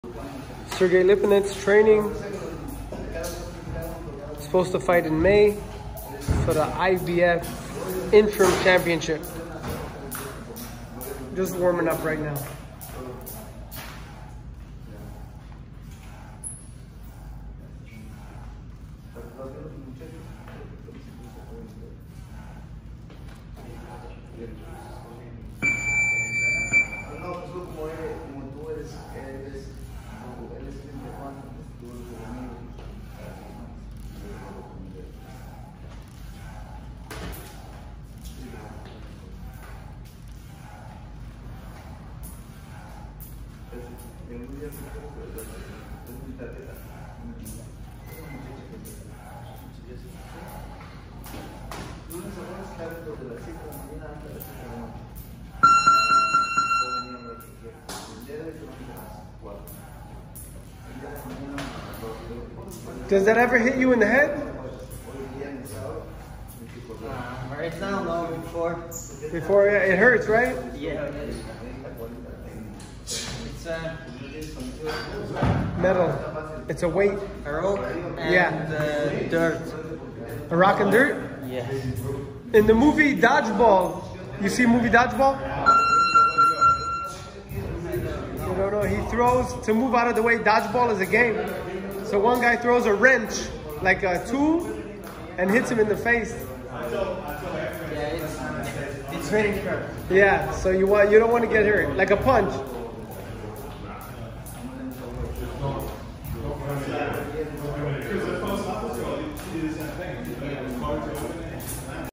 Sergei Lipinets To Fight For 147 Title
Sergei Lipinets has a title fight coming up May 16: the interim IBF World Welter Title. Kudratillo Abdukakhorov stands between him and the gold.